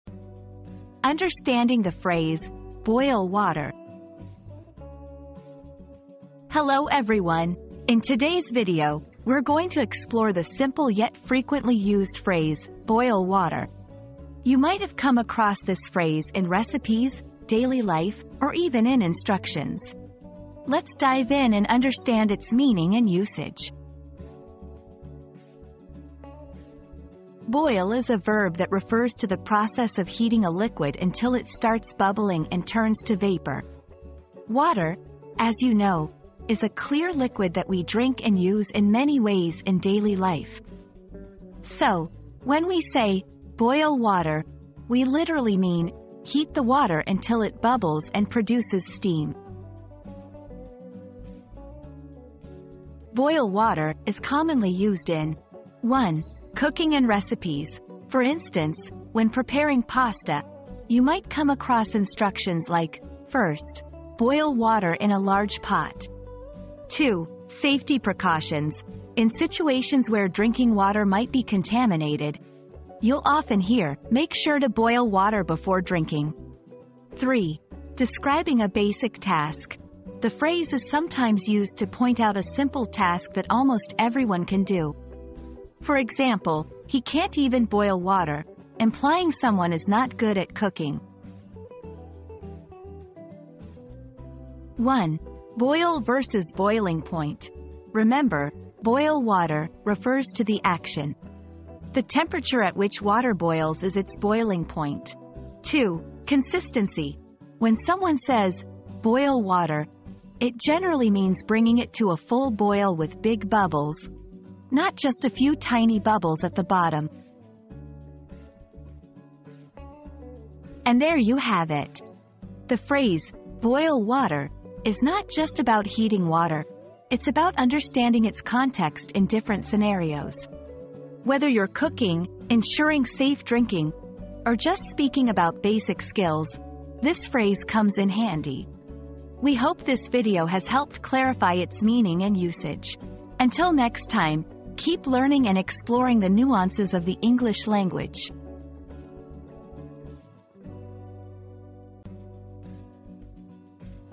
Understanding the Phrase "Boil Water"
Understanding the Phrase 'Boil Water' • Discover the true meaning and usage of the commonly used phrase 'boil water.' From its literal definition to its context in cooking, safety precautions, and everyday language, learn about common mistakes and tips for using this phrase correctly.

00:00 • Introduction - Understanding the Phrase "Boil Water"
00:26 • Literal Meaning
00:52 • Usage in Context
01:34 • Common Mistakes & Tips